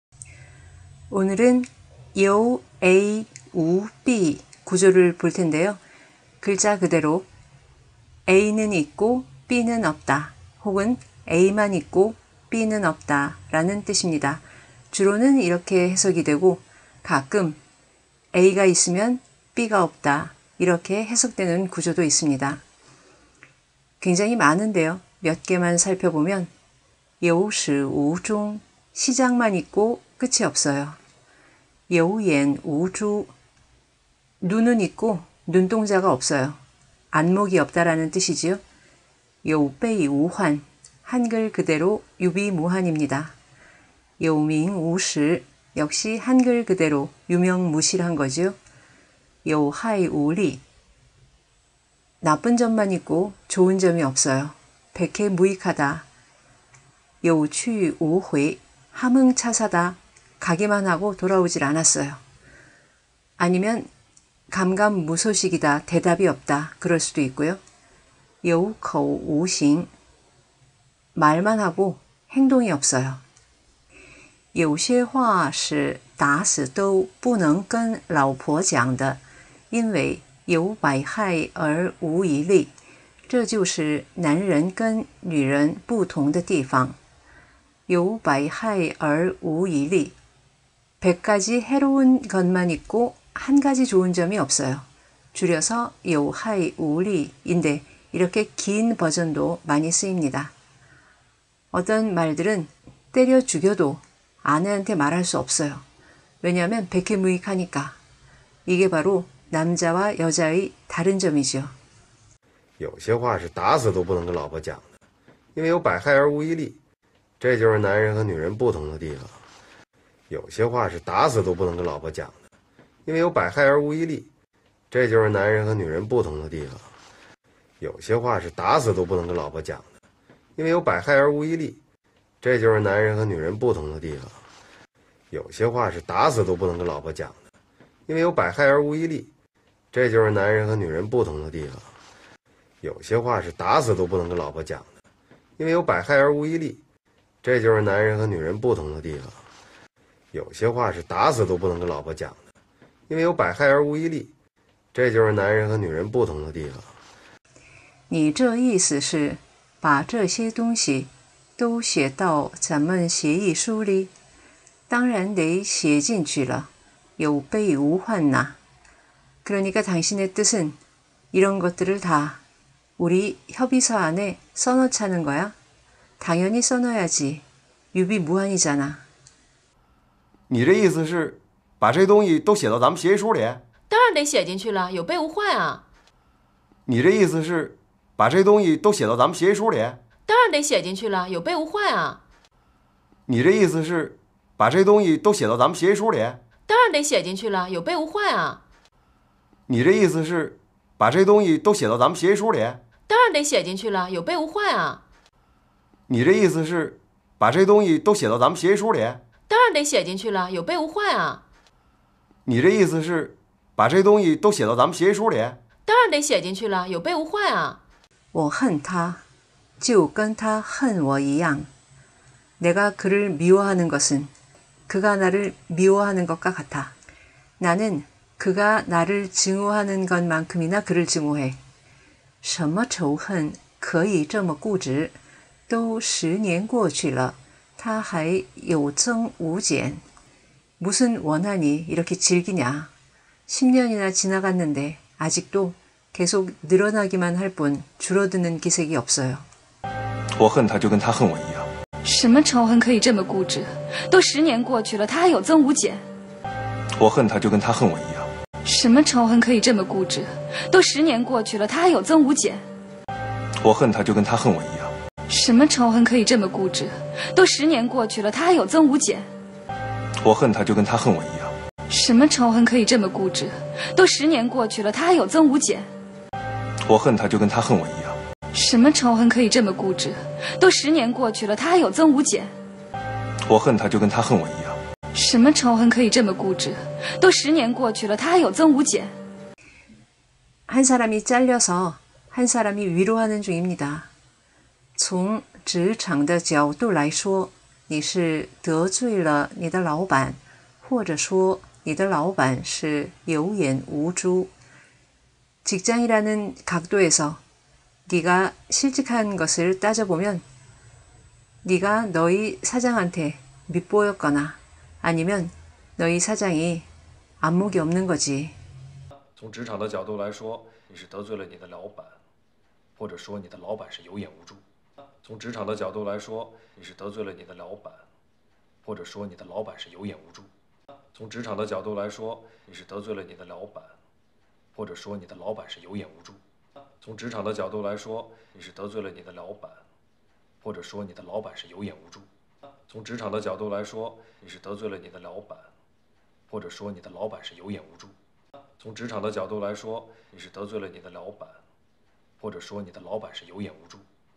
A만 있고 B는 없다 有A无B 중국드라마 대사로 배우기
A만 있고 B는 없다는 뜻의 有A无B의 뜻을 배우고 드라마 대사로 반복해서 외워봅니다